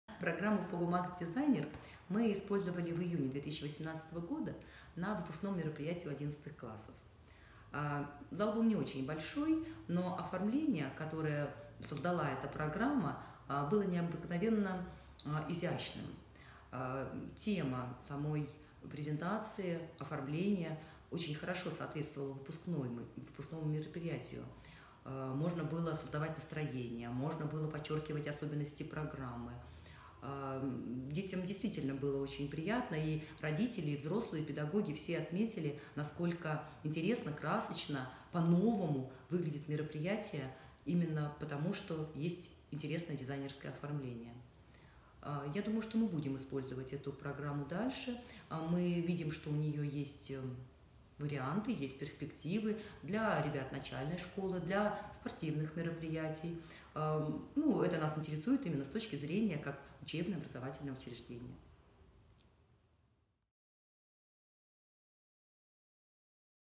Александра Вильман. Отзыв о программе POGUMAX Designer [POGUMAX]
В июне 2018 года команда POGUMAX участвовала в оформлении выпускного вечера в гимназии №8 (г. Новосибирск).

Отзыв директора о применении программы POGUMAX Designer.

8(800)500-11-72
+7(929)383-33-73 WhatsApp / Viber